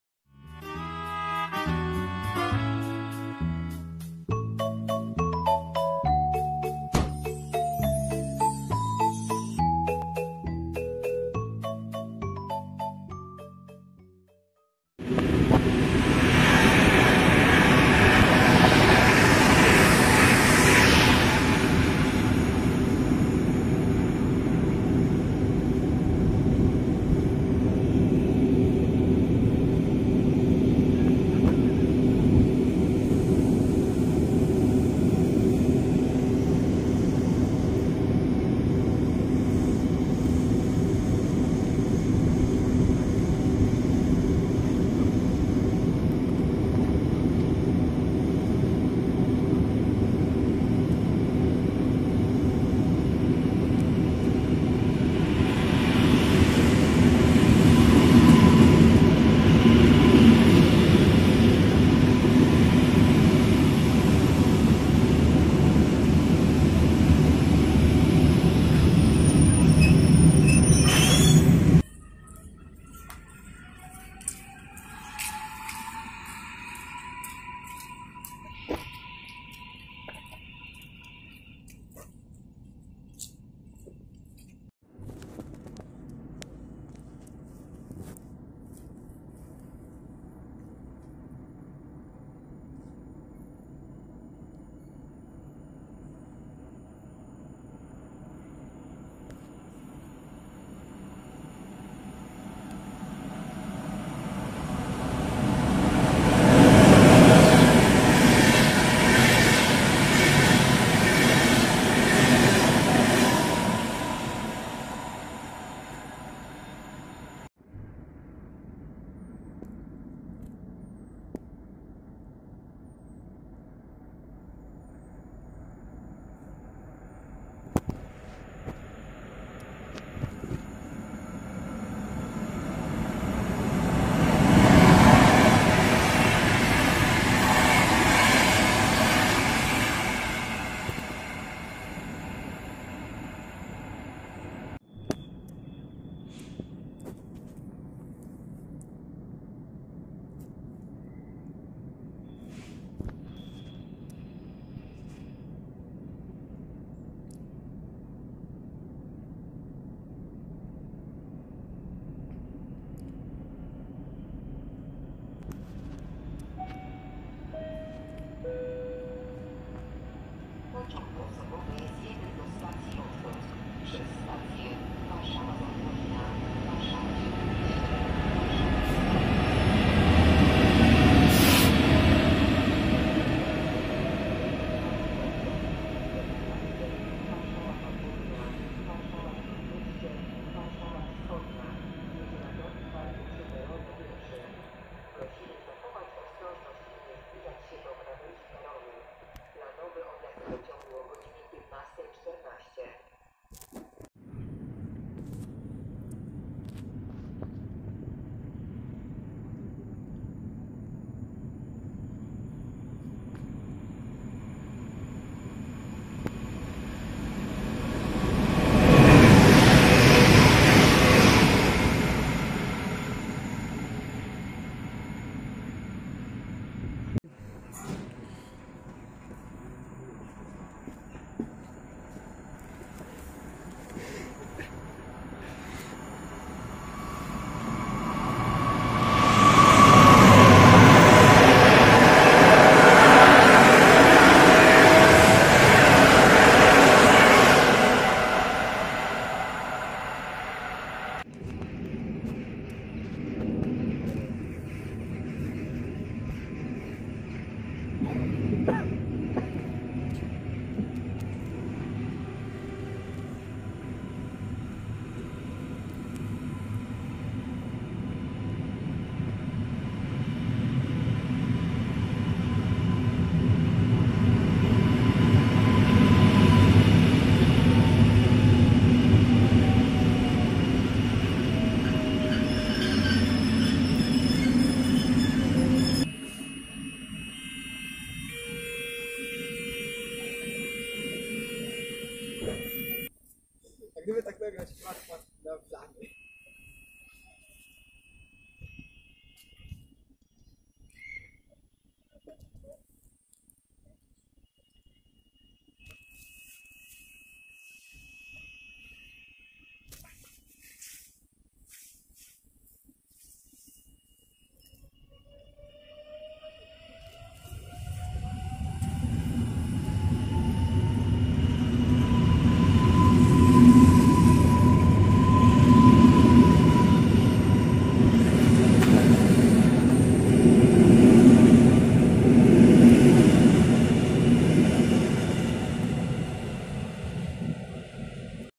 Składanka kolejowa z MikuSiem Pruszków 20.01.2019
Pozdrowienia dla MikuS i dzięki za spotkanie

Miejsce nagrania: Pruszków, Linia Kolejowa D29-1, D29-447 i D29-512